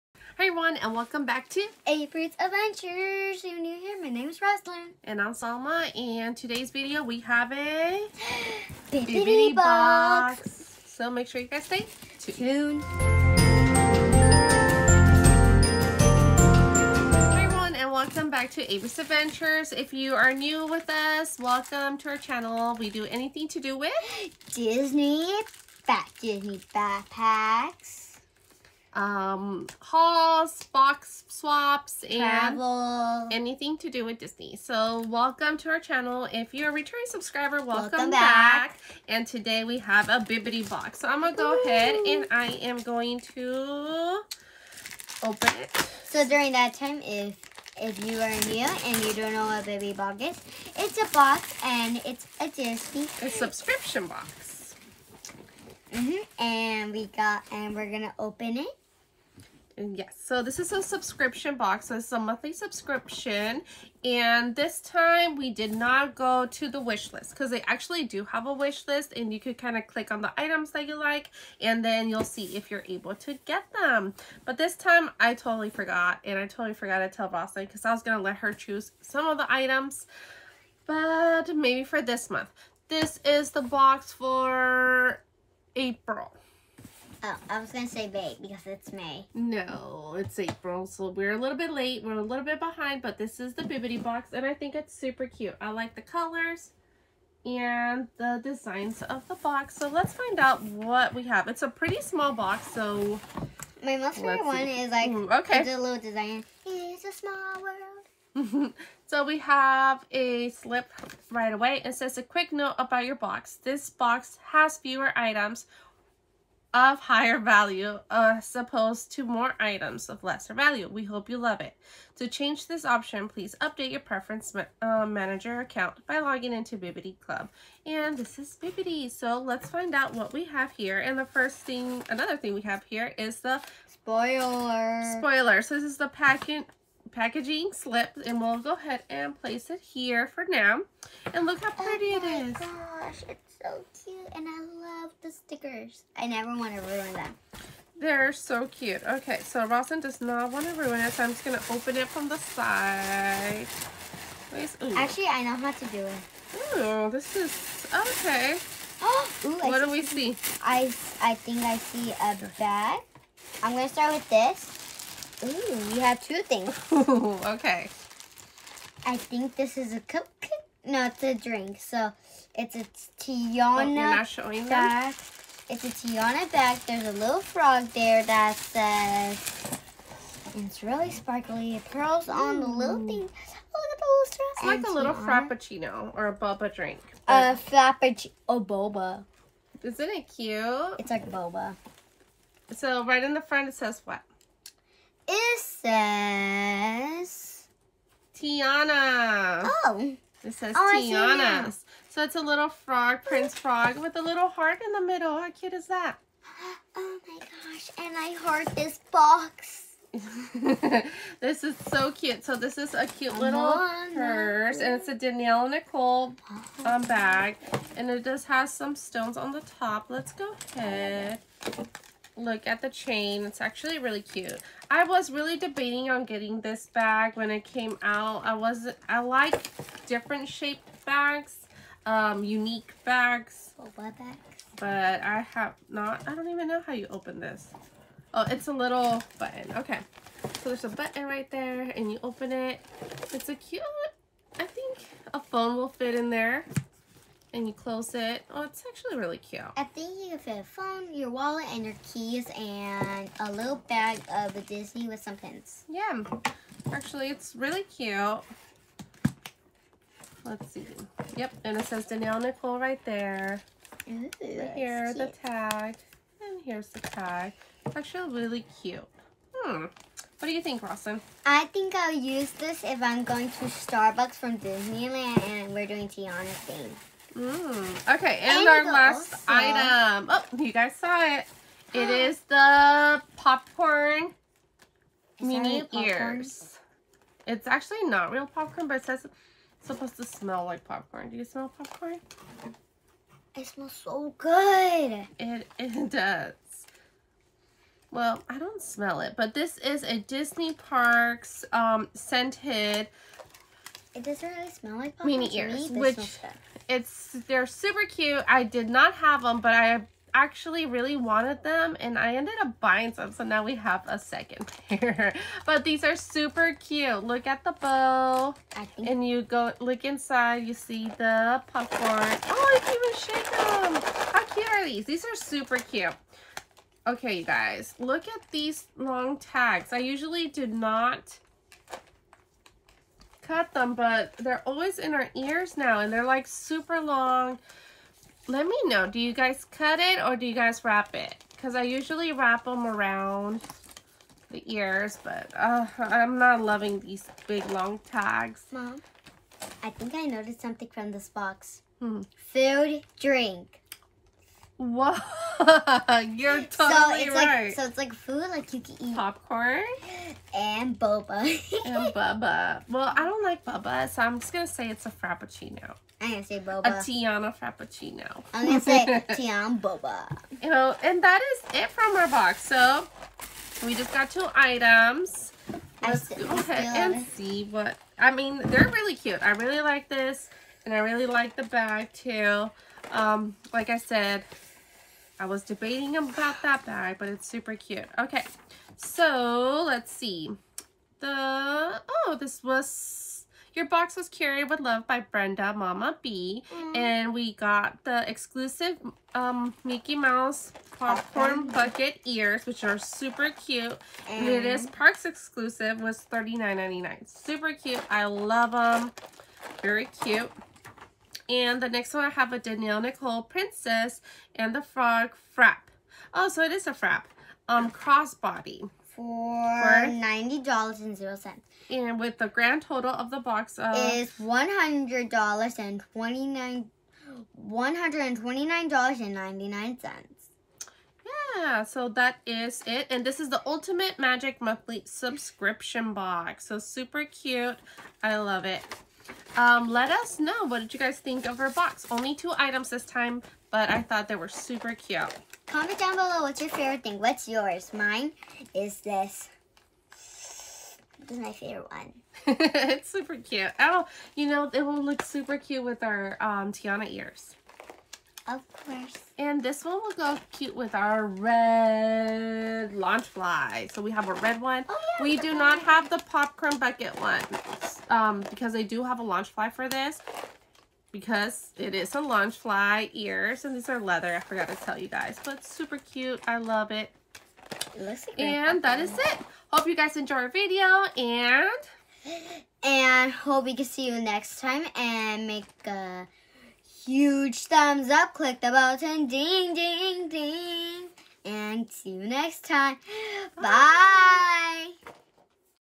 Bibbidi Unboxing | April Subscription box
Only Two items this time in our Ultimate Magic box! Come and check out what we got and was its worth the $119 subscription !

We love Magic Mail:
Abri's Adventures